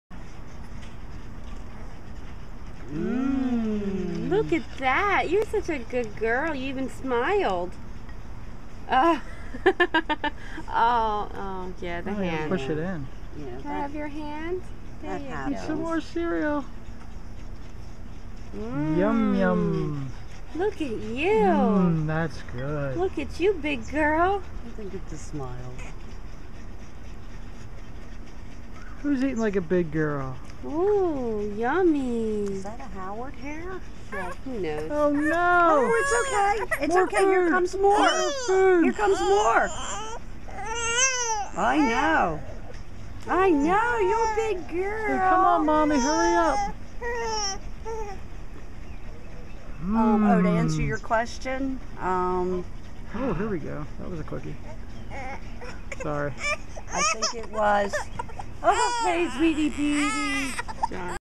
Ava Eating
My daughter eating some cereal.